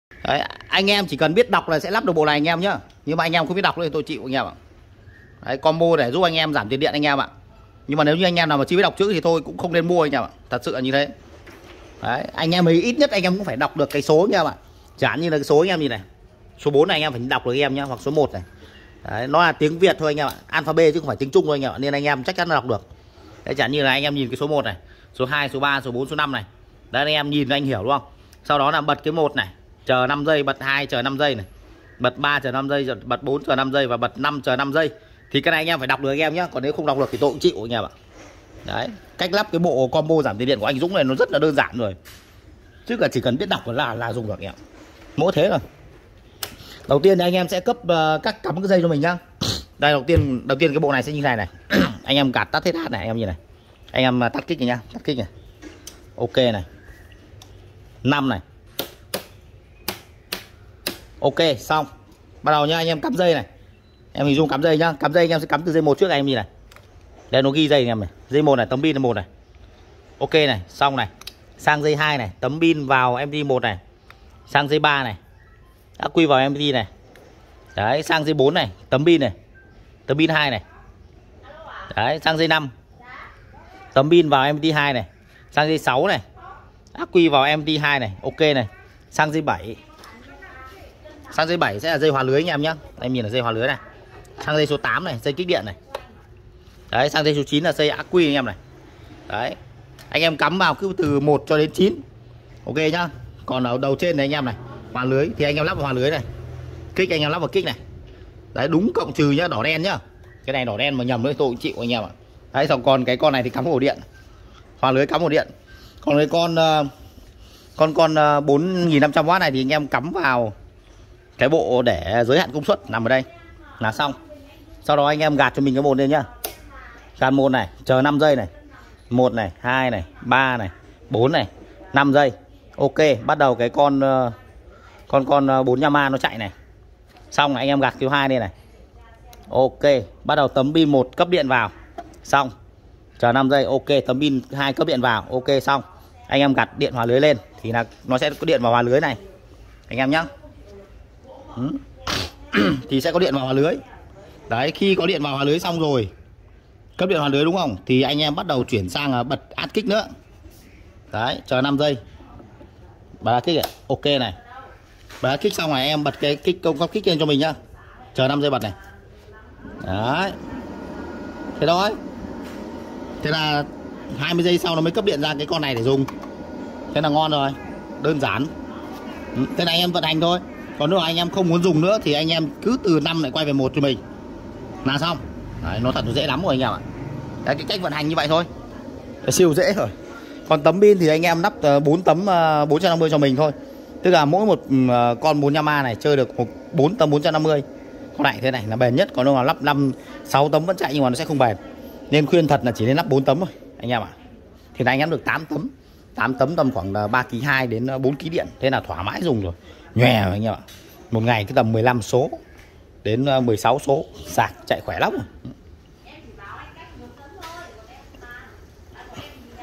Combo giảm tiền điệnCực kì hiệu quảMất điện chạy điều hòa, tủ lạnh, máy giặt2 sạc thần thánh
Combo giảm tiền điện
Cực kì hiệu quả
Mất điện chạy điều hòa, tủ lạnh, máy giặt
2 sạc thần thánh
1 tủ tổng
1 hòa lưới 2000w
 kích điện 4500w
bình 48v 280Ah